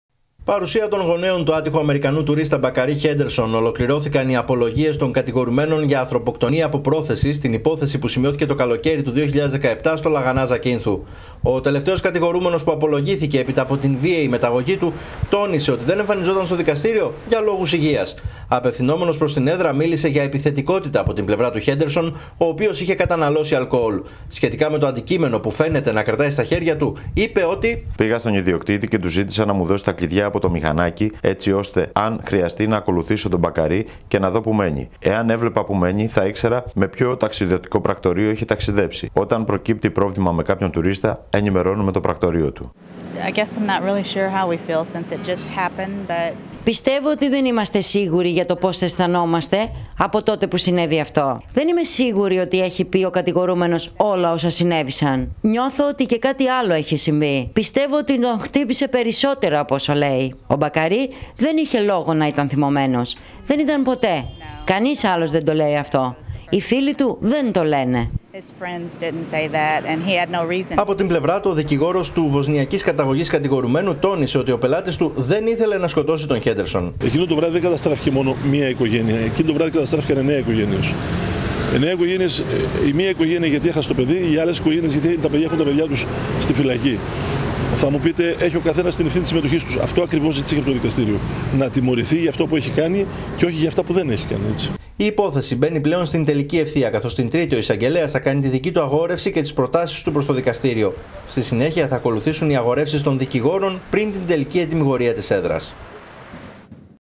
ΠΑΤΡΑ | Απολογήθηκε ο Βόσνιος στην δίκη Χέντερσον. Στο δικαστήριο οι γονείς του αμερικανού
Από τα χείλη του Εισαγγελέα κρέμονται τώρα, κατηγορούμενοι και οι γονείς και οι φίλοι του Μπακαρί Χέντερσον, καθώς ολοκληρώθηκαν οι απολογίες των κατηγορουμένων, για το αιματηρό περιστατικό που σημειώθηκε το 2017 στο Λαγανά Ζακύνθου, με θύμα τον 22χρονο Αμερικανό τουρίστα. Στο Μικτό Ορκωτό Δικαστήριο της Πάτρας, βρέθηκαν οι γονείς του Μπακαρί Χέντερσον, ενώ ο Εισαγγελέας θα αγορεύσει την Τρίτη, οπότε και θα ανακοινώσει την πρότασή του